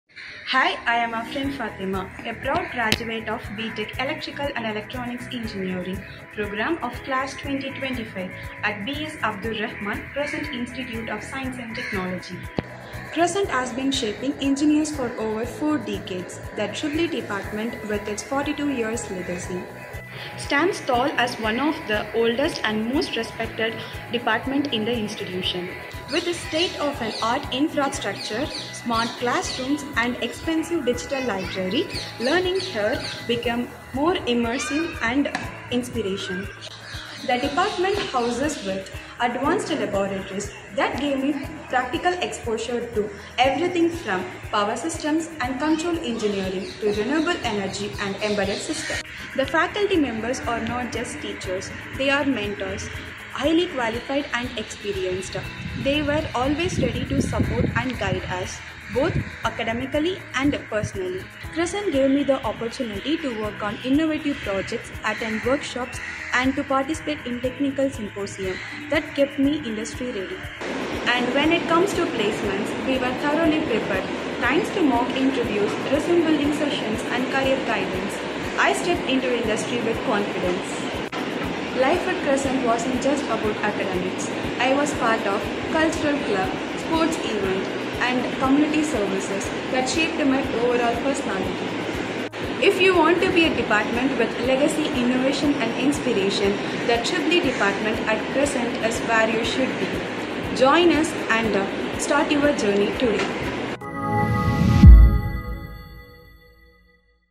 Ms.Afrin Fathima H | Hitachi Energy Alumni (Batch 2021-2025) | EEE | Student testimonial
Ms.Afrin Fathima H | Hitachi Energy Alumni (Batch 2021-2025) | EEE
| Student testimonial

ADMISSIONS OPEN 2025
Admission Helpdesk:
B.S.Abdur Rahman Crescent Institute of Science & Technology,
GST Road, Vandalur, Chennai 600 048.